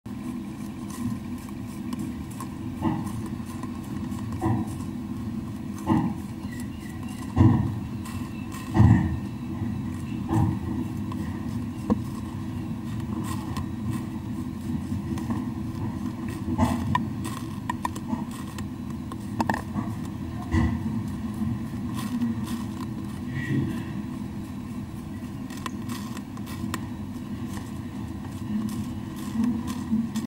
Tomato Horn Worm tearing through potato leaf
This huge Horn worm ate through a whole potato plant in one night! Unbelievable, I know, but look at him go and you see how he does it.